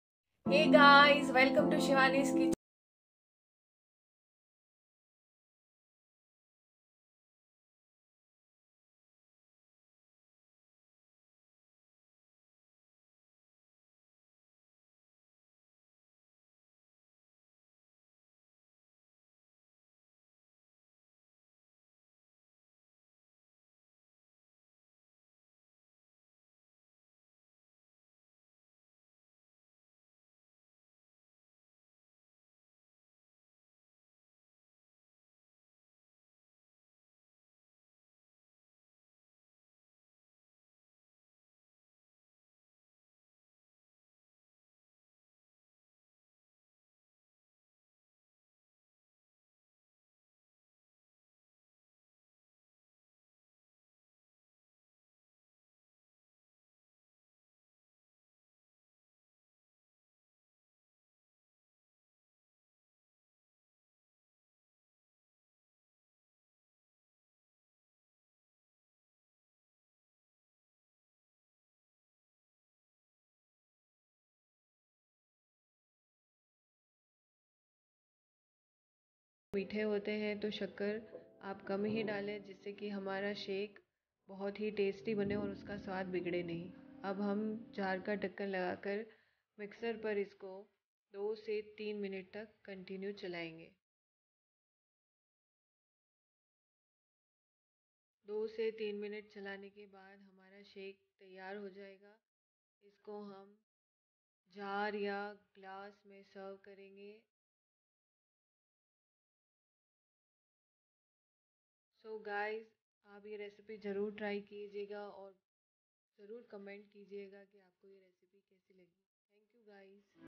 chikoo shake/Sapota milkshake /5mnts recipe
सामग्री: तीन से चार चीकू को तीन चम्मच मिल्क क्रीम एक कप दूध एक चम्मच चीनी!
चीकू शेक बनाने की विधि: चीकू को छीलकर मिक्सर के जार में लेंगे, चीकू के बीज निकालना ना भूले! दो-तीन चम्मच क्रीम डालेंगे! एक कप दूध डालेंगे ध्यान रहे कि दूध ठंडा हो अगर दूध ठंडा ना हो तो दो से तीन बर्फ के टुकड़े भी डाल सकते हैं! चीनी डालें मिक्सर के जार के ढक्कन को लगाकर मिक्सर को दो से 3 मिनट तक चलाना है! 3 मिनट होने के बाद हमारा चीकू शेक तैयार हो जाएगा!